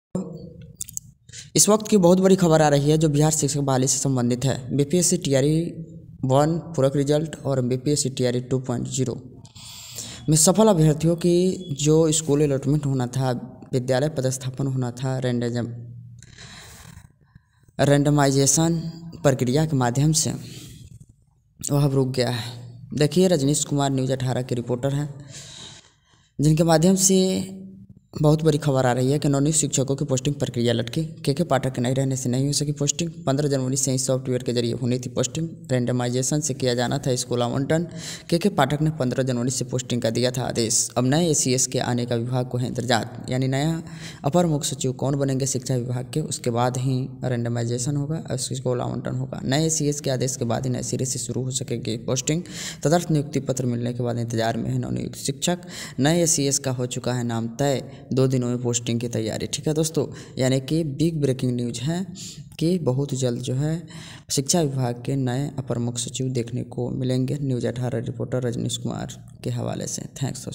बिहार शिक्षक बहाली(BPSC TRE 2.0):स्कूल आवंटन प्रक्रिया रूकी,लेकिन क्यों?शिक्षा विभाग का नये ACS कौन?
बिहार सरकार#के के पाठक#शिक्षा विभाग बिहार शिक्षक बहाली(BPSC TRE 2.0):स्कूल आवंटन प्रक्रिया रूकी,लेकिन क्यों?शिक्षा विभाग का नये ACS कौन?